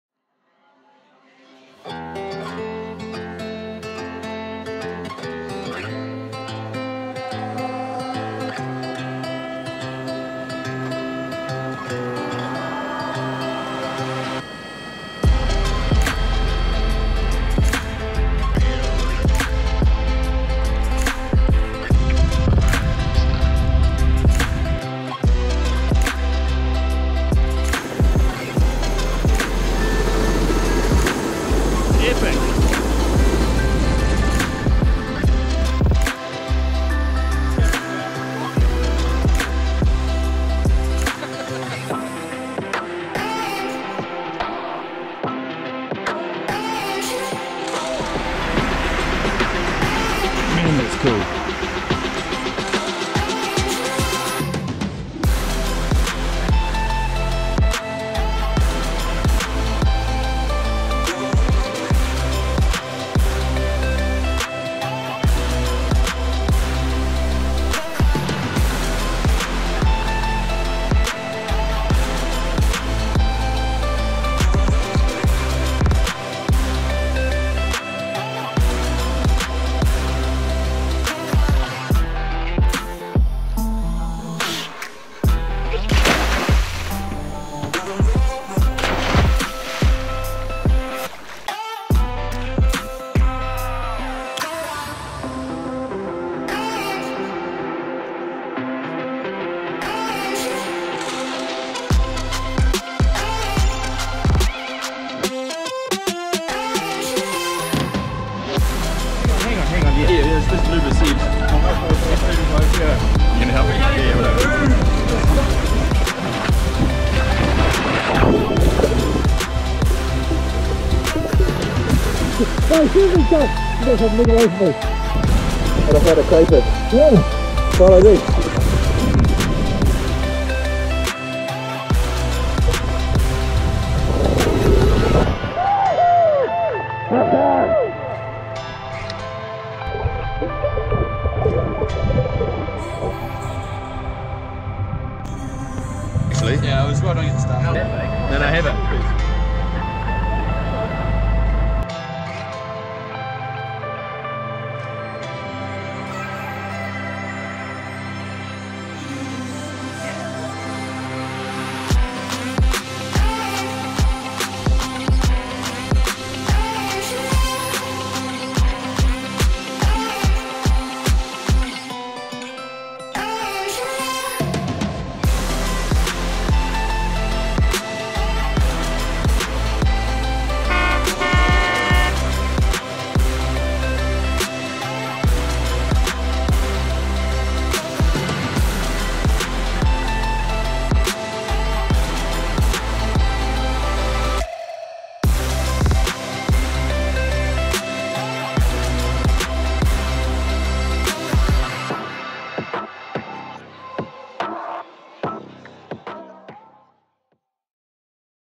Pure Salt Winter Wonders
Fiordland is stunning year around for different reasons.. winter brings less rainfall, still waters, stunning sunrises and sunsets, crazy water clarity, amazing photography opportunities and another layer of calm..